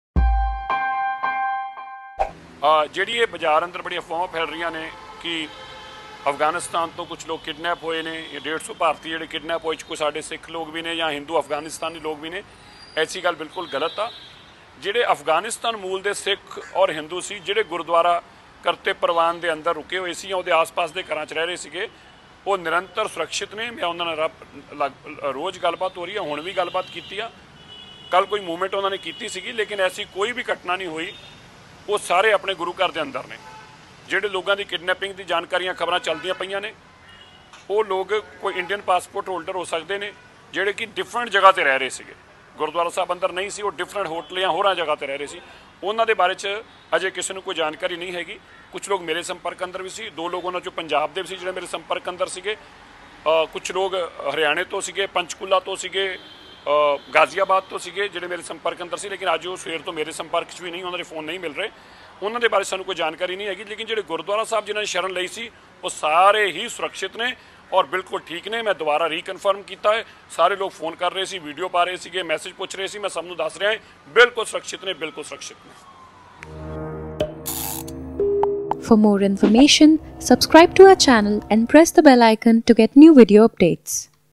All Indians living in and around gurudwara in Afghanistan are safe, no one kidnapped | cobrapost
@CobraPostIndia Hours after reports of around 150 Indian citizens been kidnapped by the Taliban Saturday morning, Manjinder Singh Sirsa, President of Delhi Sikh Gurdwara Management Committee clarified that around 300 Sikhs and Hindus who are in the gurudwara premises in Afghanistan are safe.